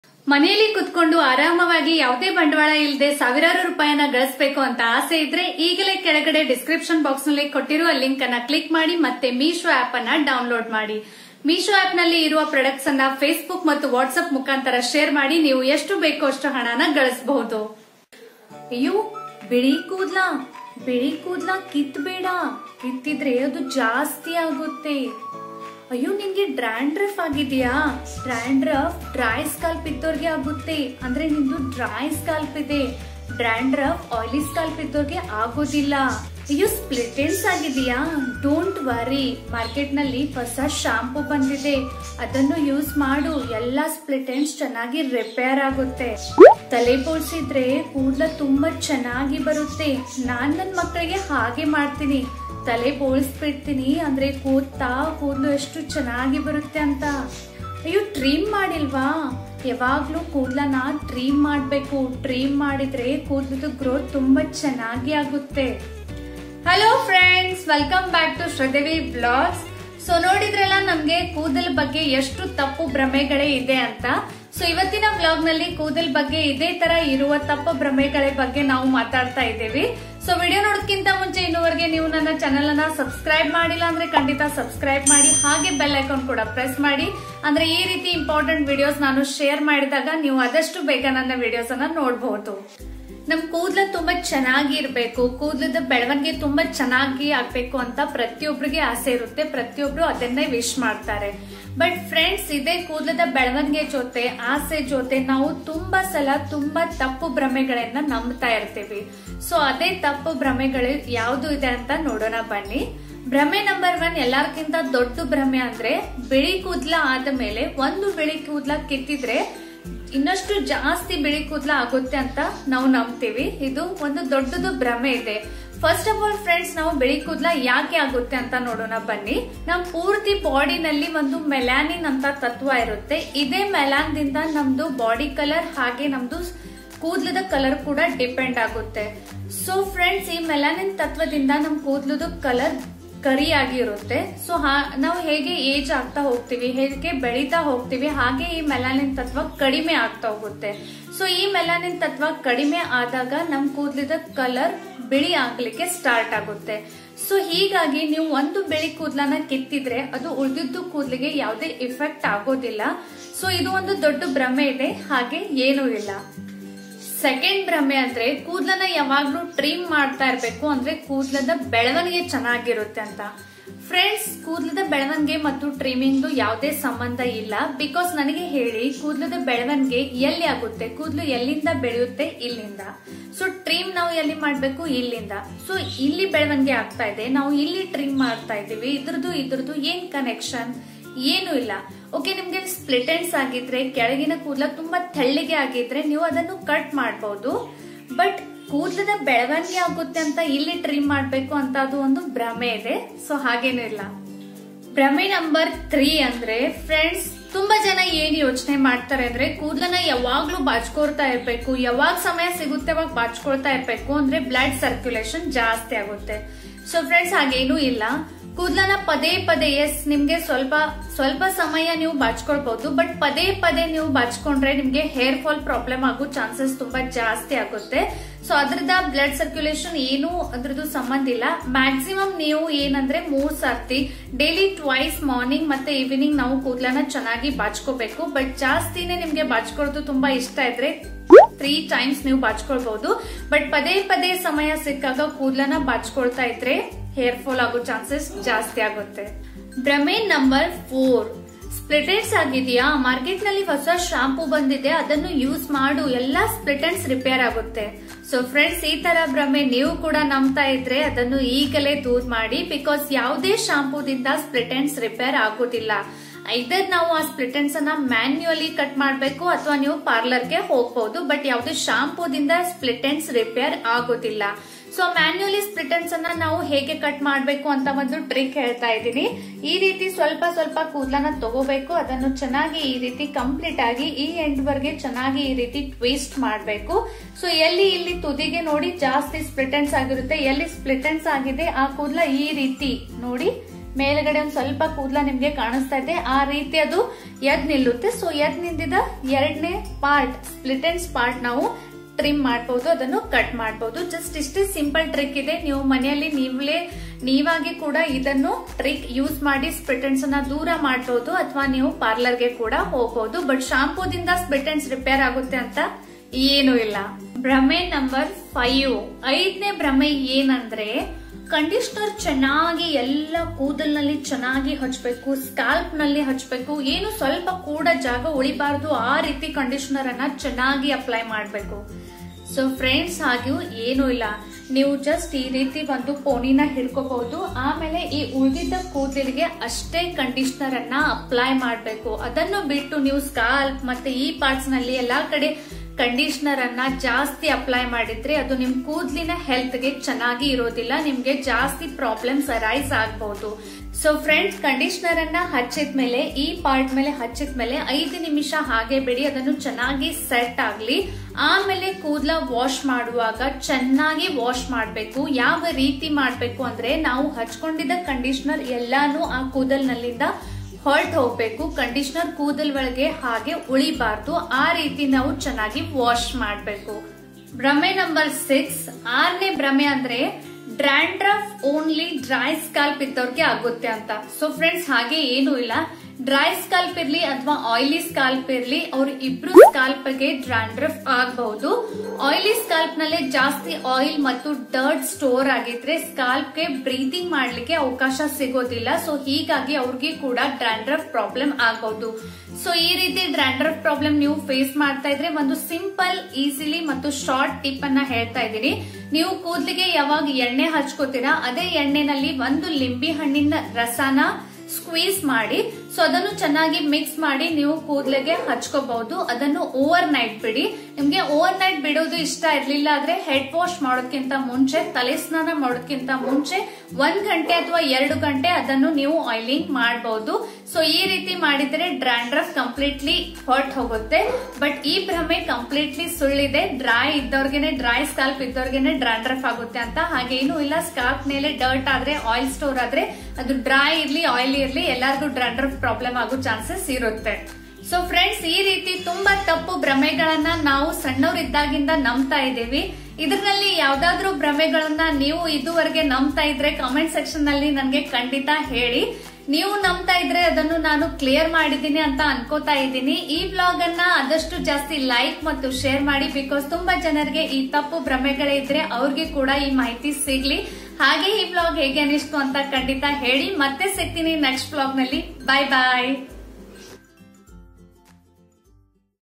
ಕೂದಲಿನ ಬಗ್ಗೆ ಇರುವ ತಪ್ಪು ಕಲ್ಪನೆಗಳು Hair Myths in Kannada Shridevi Vlogs Hair Care Tips
ಇಂದೇ Meesho App download ಮಾಡಿ ಮತ್ತು ಹಣ ಗಳಿಸೋಕೆ ಶುರು ಮಾಡಿ. ಈ ಲಿಂಕ್ ಕ್ಲಿಕ್ ಮಾಡಿ

ಈ ವಿಡಿಯೋದಲ್ಲಿ ನಾನು ಕೂದಲಿನ ಬಗ್ಗೆ ಇರುವ ತಪ್ಪು ಕಲ್ಪನೆಗಳ ಬಗ್ಗೆ ಹೇಳಿದ್ದೇನೆ. ವಿಡಿಯೋ ಪೂರ್ತಿಯಾಗಿ ನೋಡಿ.

So today I have shared Hair Myths in Kannada, which is not true.